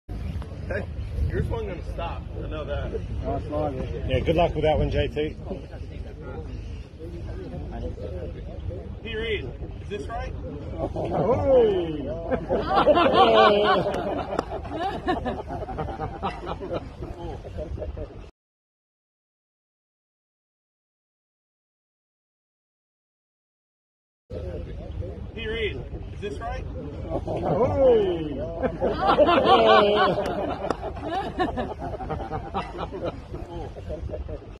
Justin Thomas hilariously trolling Patrick Reed about cheating in the 2019 Hero World Golf Challenge
All in good fun, I'm sure...

and research. In this case, these rights are most likely reserved for the PGA of America and/or the PGA European Tour Productions: respectively,